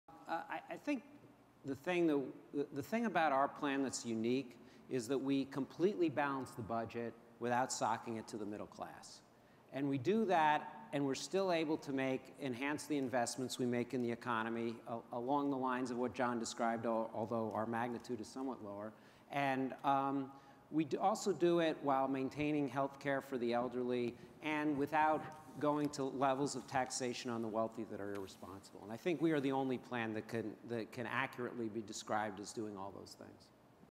Michael Ettlinger: Center For American Progress Solutions Initiative Plan | The 2011 Fiscal Summit
Michael Ettlinger, Center For American Progress:
Center For American Progress Solutions Initiative Plan
The 2011 Fiscal Summit, Washington, DC